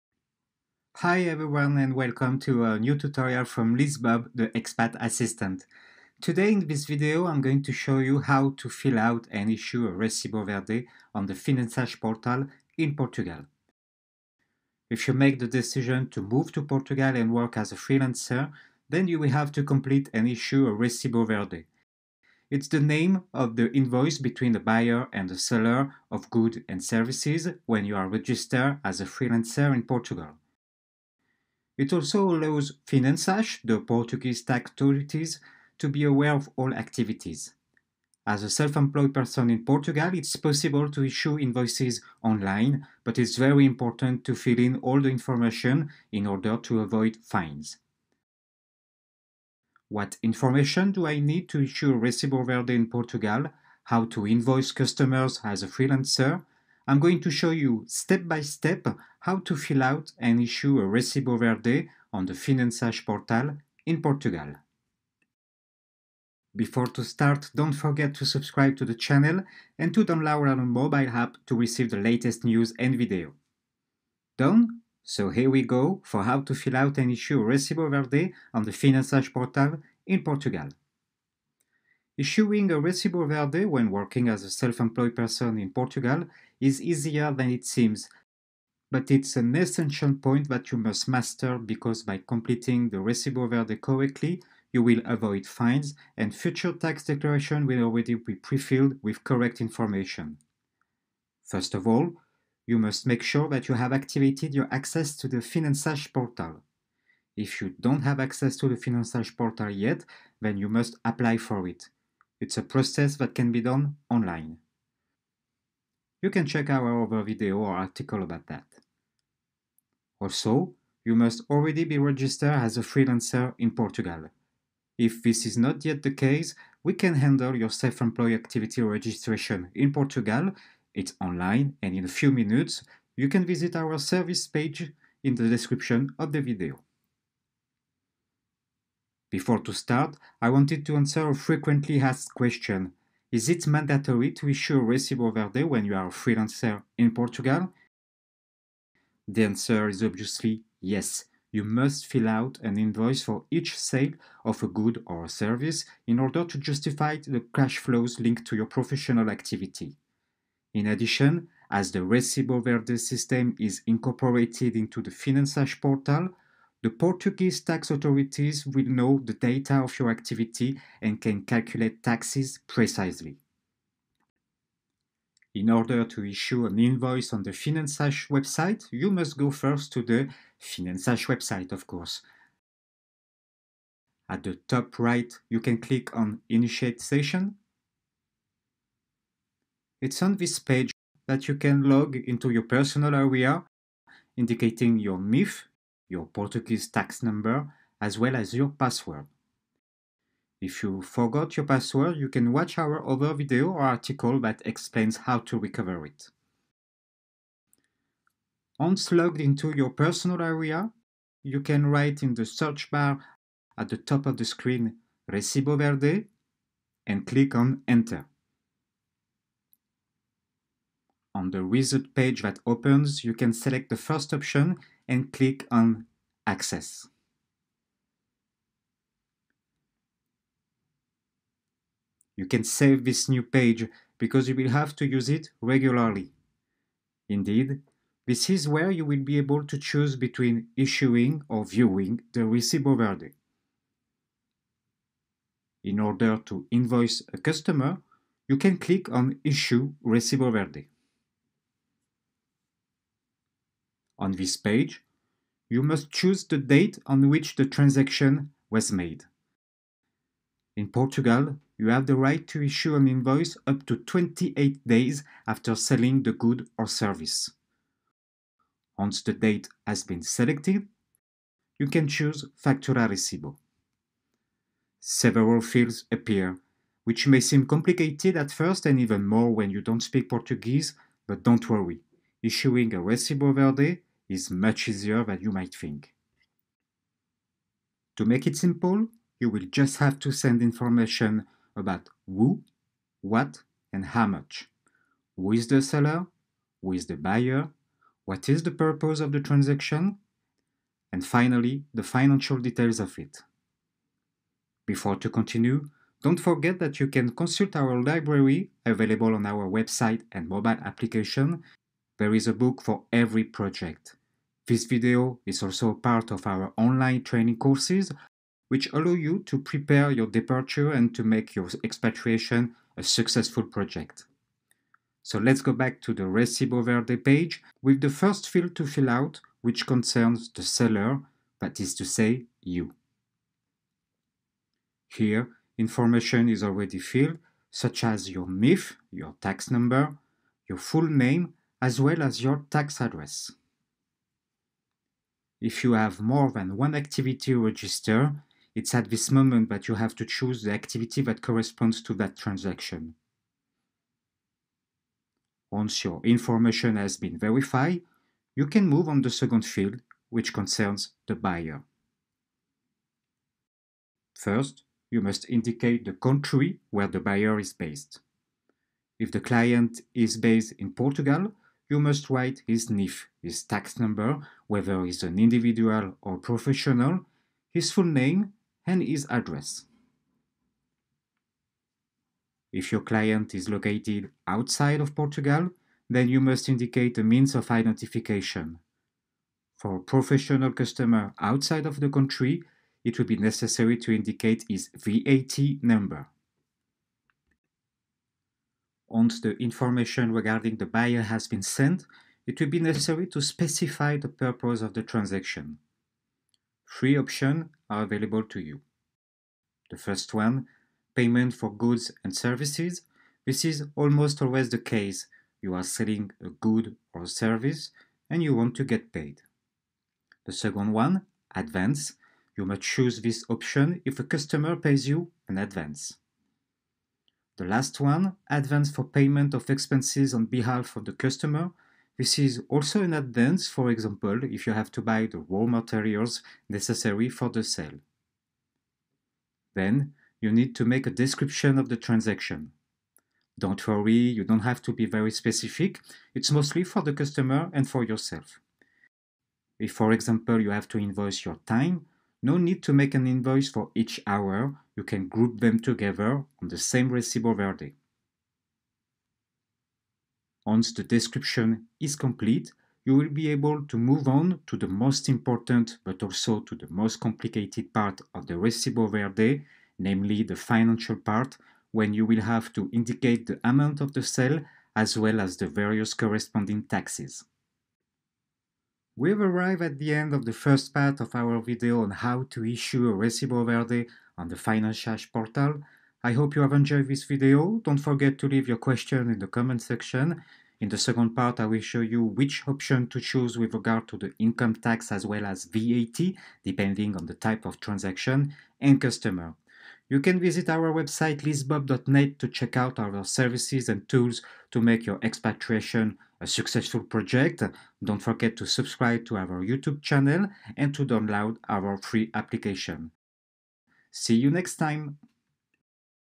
How to issue a recibo verde on Finanças portal in Portugal - Part 1
Today in this video I am going to show you how to fill out and issue a recibo verde on the Finanças portal in Portugal.

If you make the decision to move to Portugal and work as a freelancer, then you will have to complete and issue a Recibo Verde. It’s the name of the invoice between buyer and seller of goods or services when you are registered as a freelancer. It also allows Finanças, the Portuguese tax authorities, to be aware of all activities. As a self-employed person in Portugal it’s possible to issue invoices online but it’s very important to fill in all the information in order to avoid fines. What information do I need to issue a recibo verde in Portugal? How to invoice customers as a freelancer?
I will show you step by step how to fill out and issue a recibo verde on the Finanças portal in Portugal.